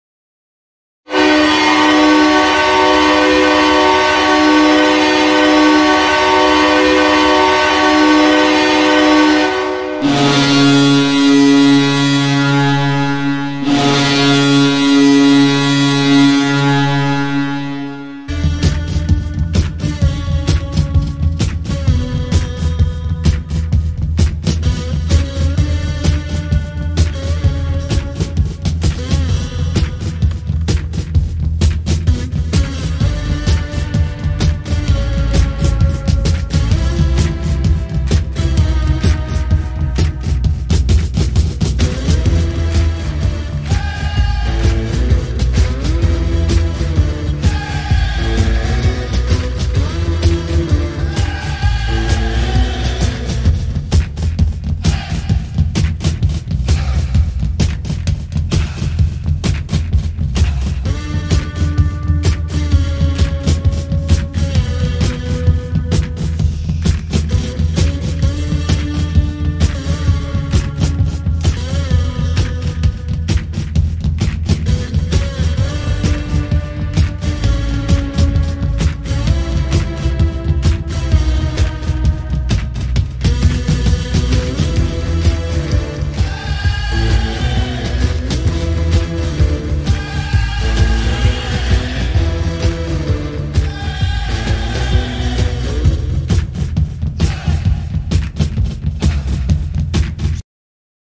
Bismarck Bobcats Goal Horn 2022 (REMADE)
This Goal Horn is used at V.F.W. Sports Center in Bismarck, ND.